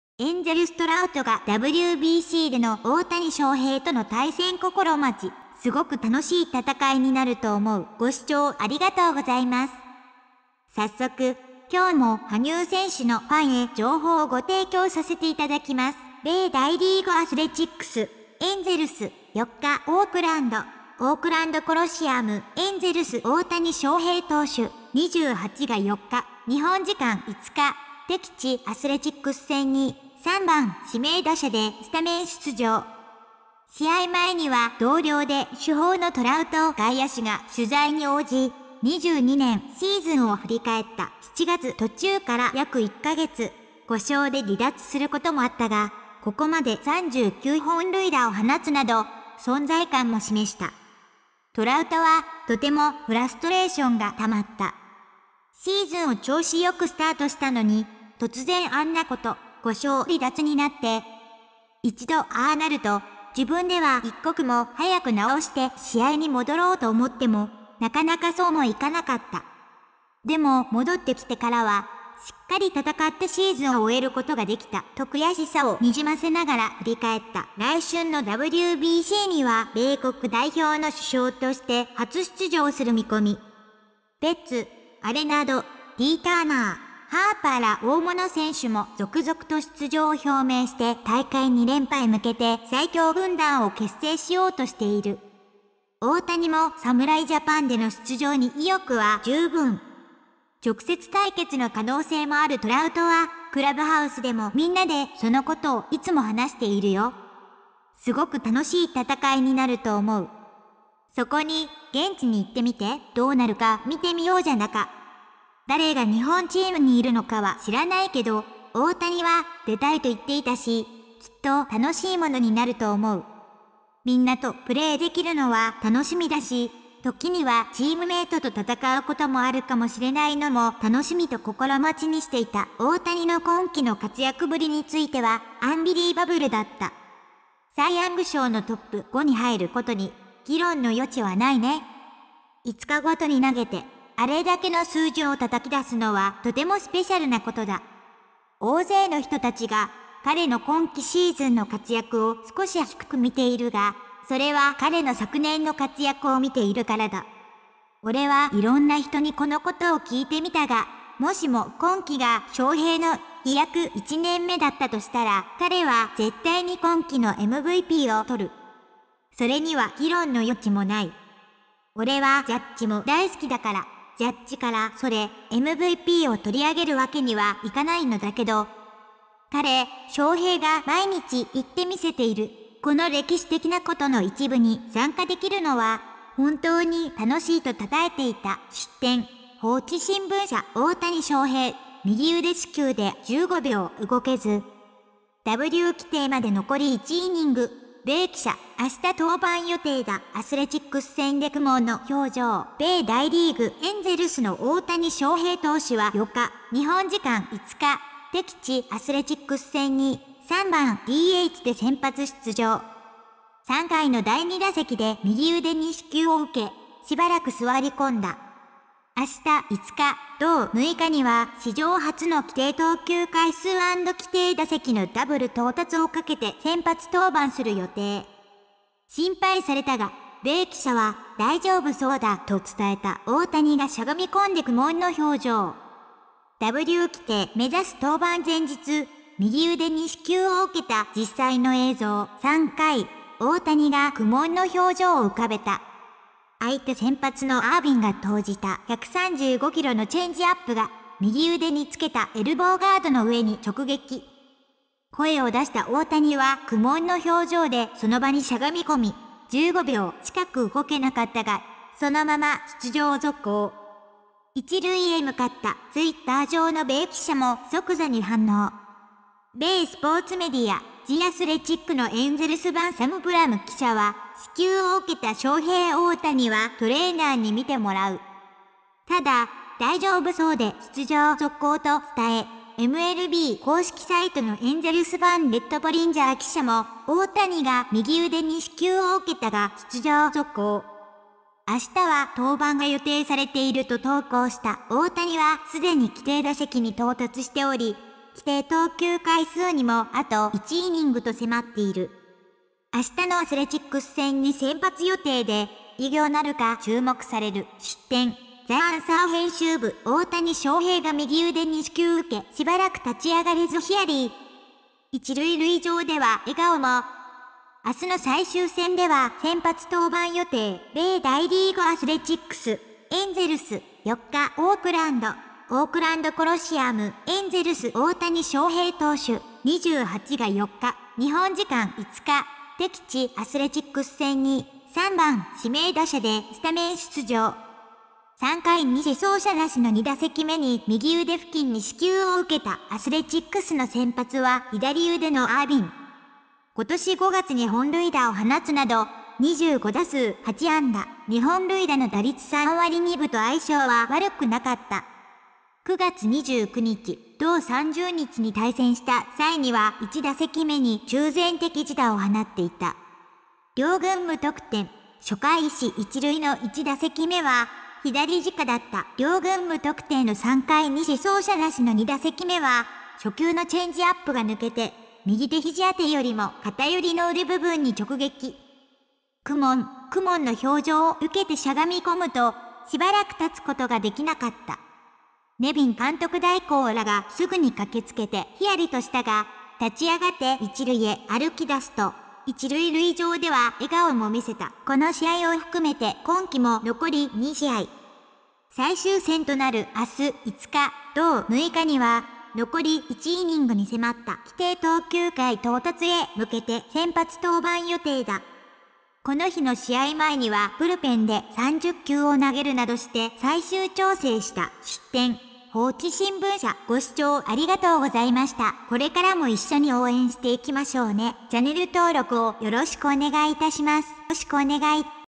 【大谷翔平】エンゼルス・トラウトがＷＢＣでの大谷翔平との対戦心待ち「すごく楽しい戦いになると思う」
MLB - 大谷翔平 ニュース・速報
大谷翔平に関する野球ニュース・速報・コラムやインタビューの記事一覧。